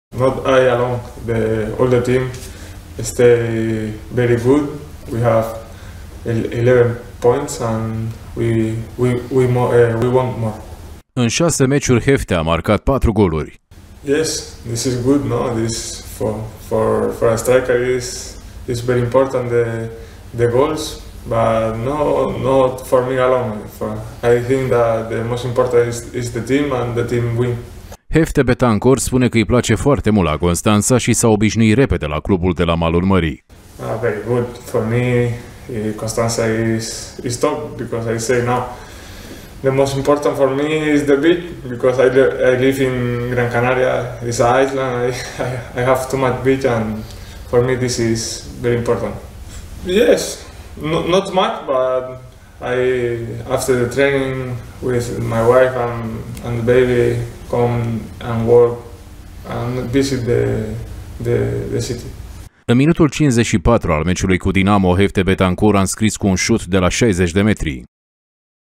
În 6 meciuri, Betancor a marcat 4 goluri
Cu cele două goluri reușite în victoria cu Dinamo, scor 3-0 în etapa a 6-a, dintre care al doilea de la 55m, Jefte Betancor a fost ales și de fanii Farului cel mai bun jucător al partidei.